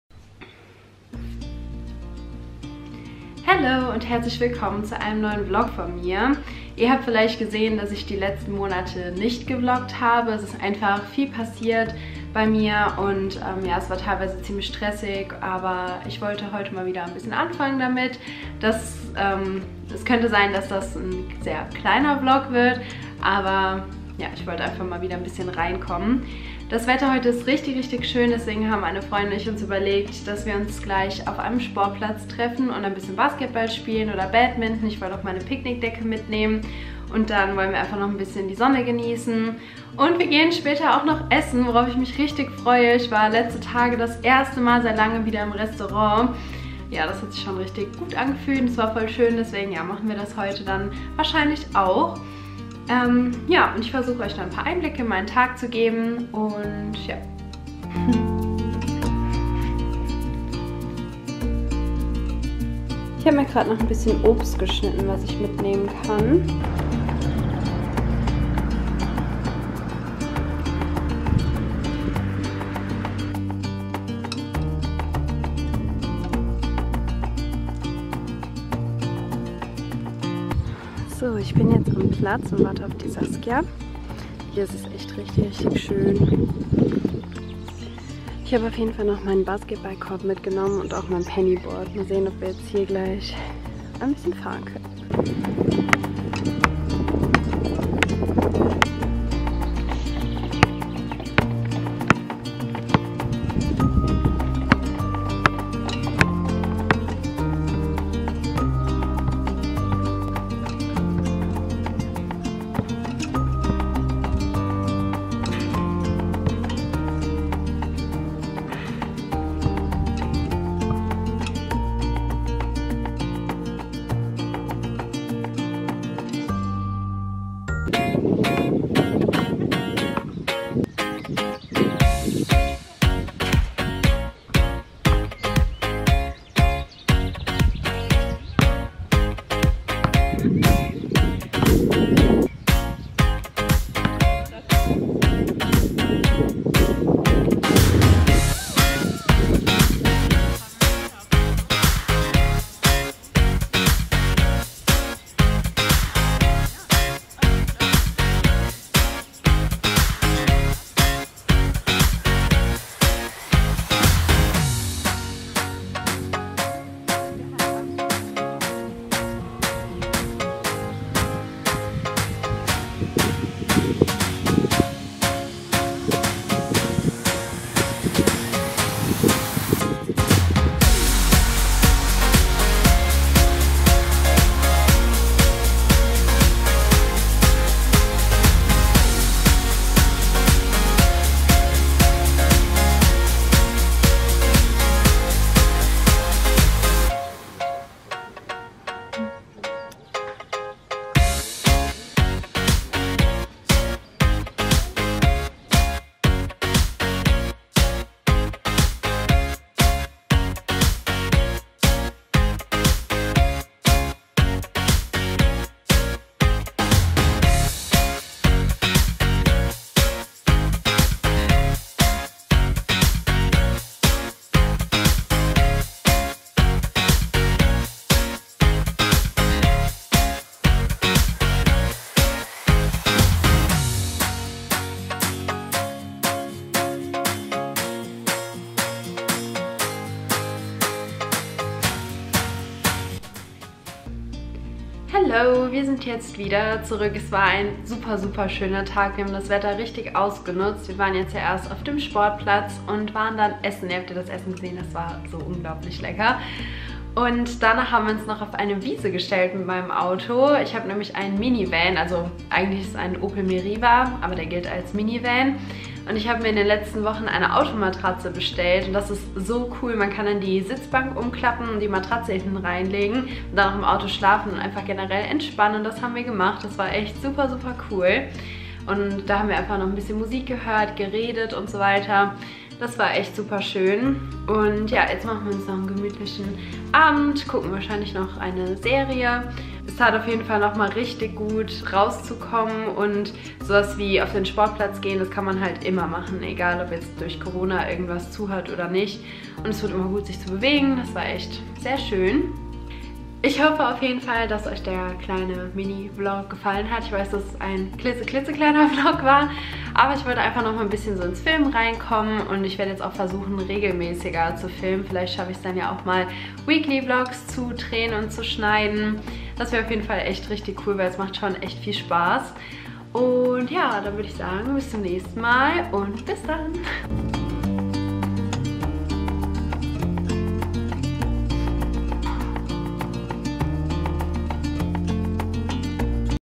vlog: outdoor fun & restaurant besuch 🍽🌿 | vivien rose
Hellooo! :)

Endlich mal wieder ein vlog!
Ich war mit einer Freundin auf einem Sportplatz , anschließend waren wir im Restaurant, finally, war so schön :)